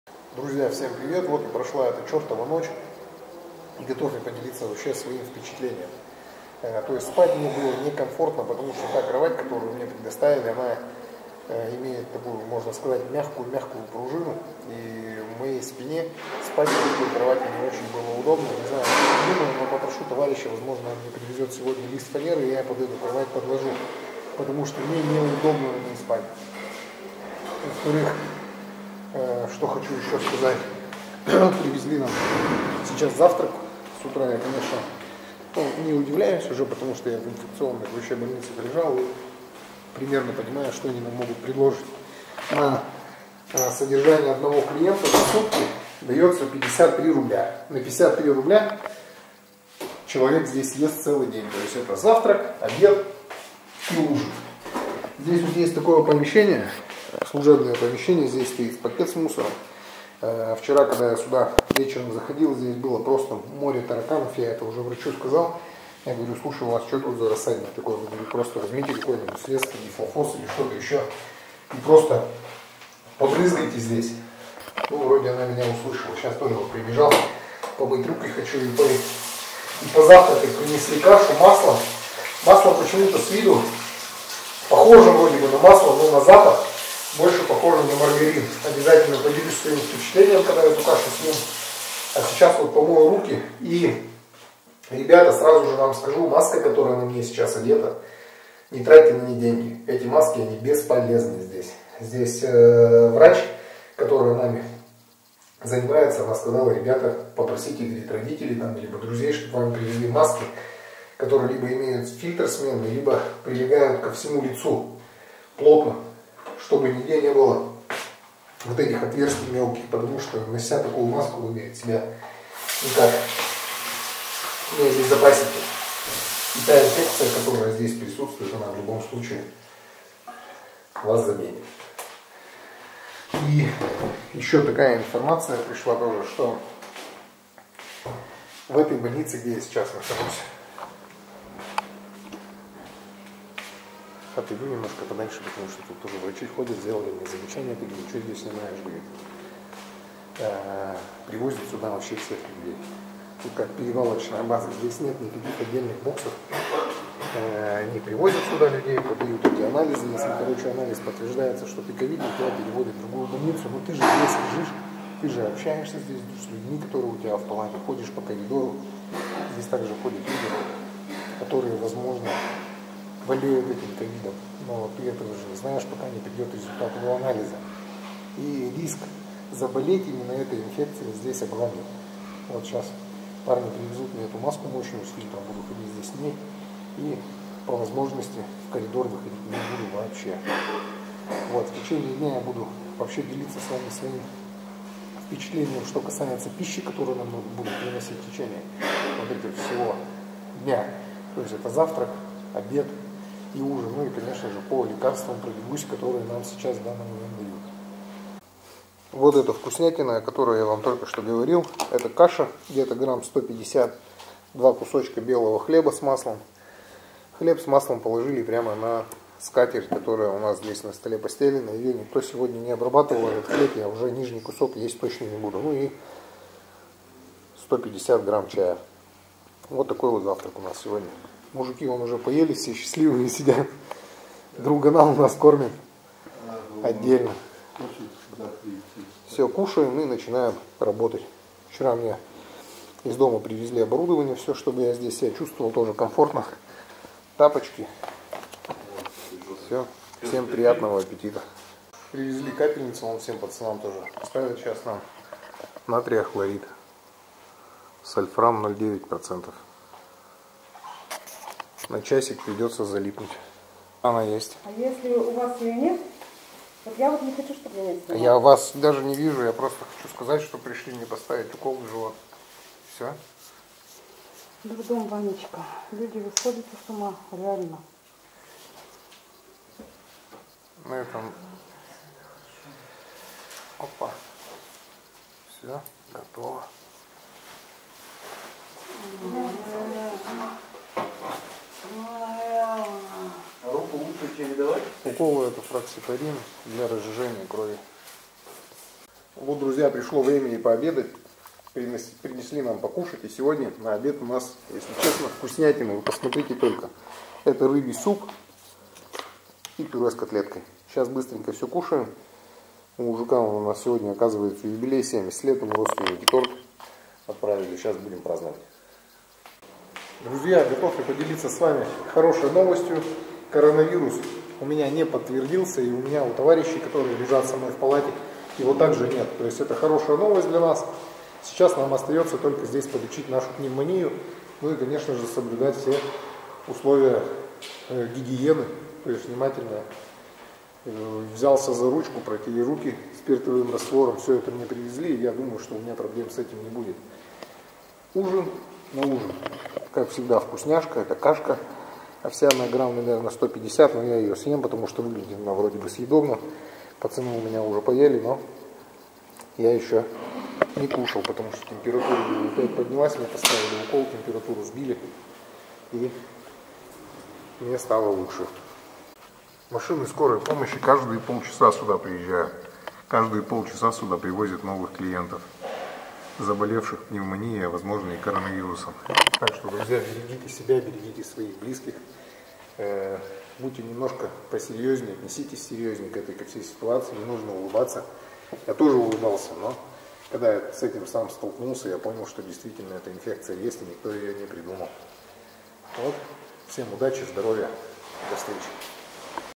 Первые впечатления от лечения и содержания в больнице
Всем привет в этом видео я хотел бы поделиться с Вами как нас кормят в больнице. Думаю выживем, если не заразимся.